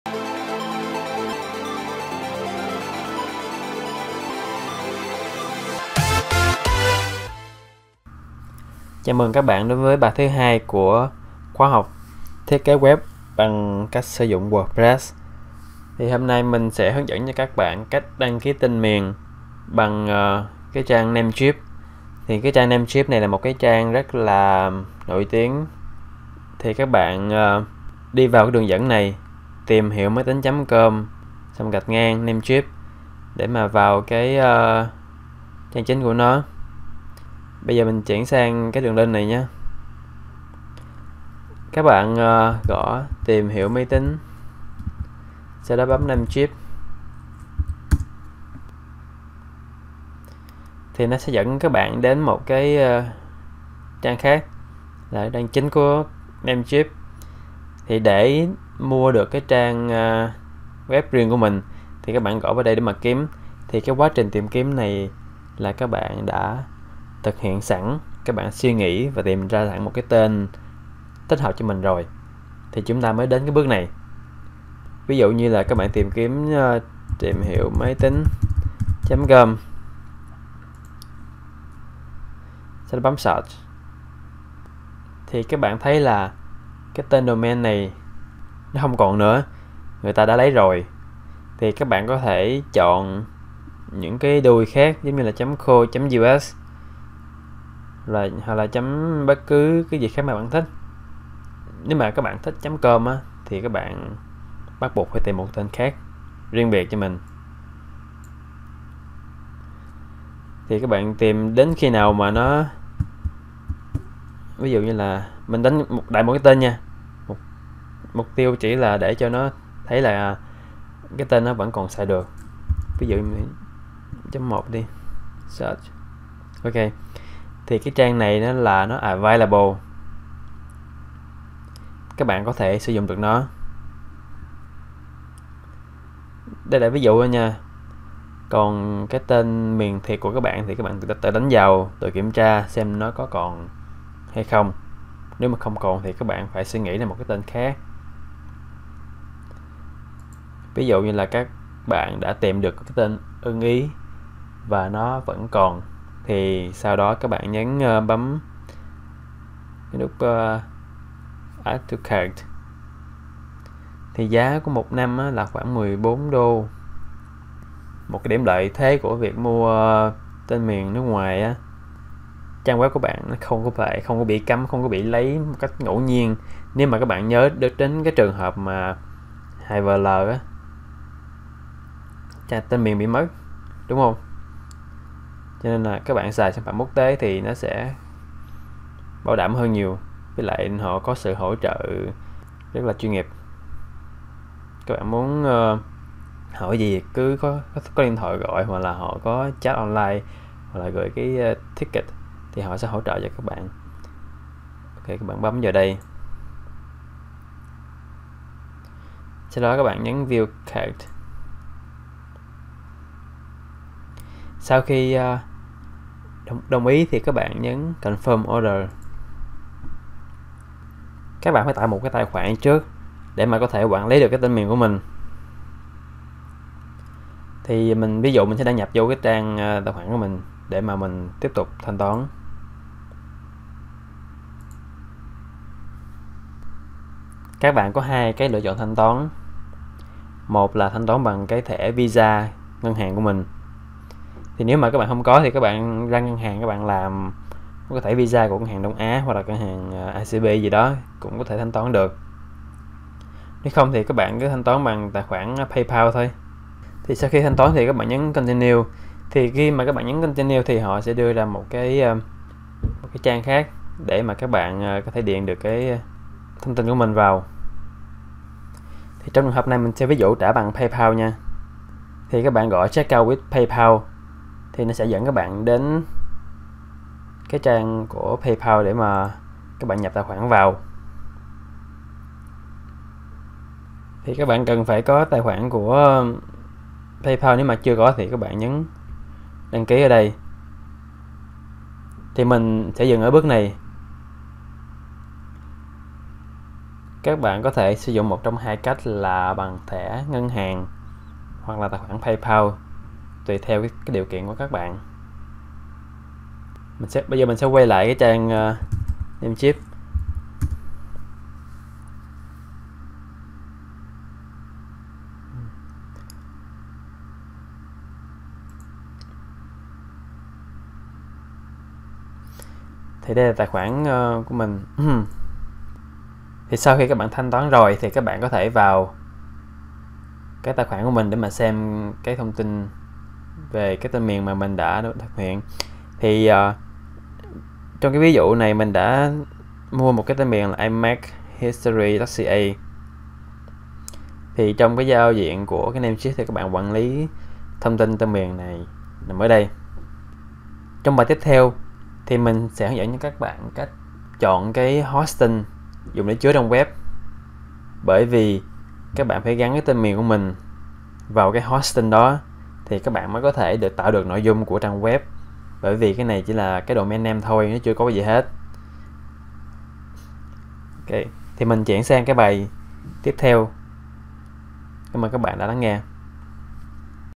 Hướng Dẫn Đăng Ký Tên Miền - Bài 2
Bài thứ 2 trong loạt bài hướng dẫn thiết kế web bằng WordPress mình sẽ hướng dẫn cho các bạn cách mua tên miền (domain name):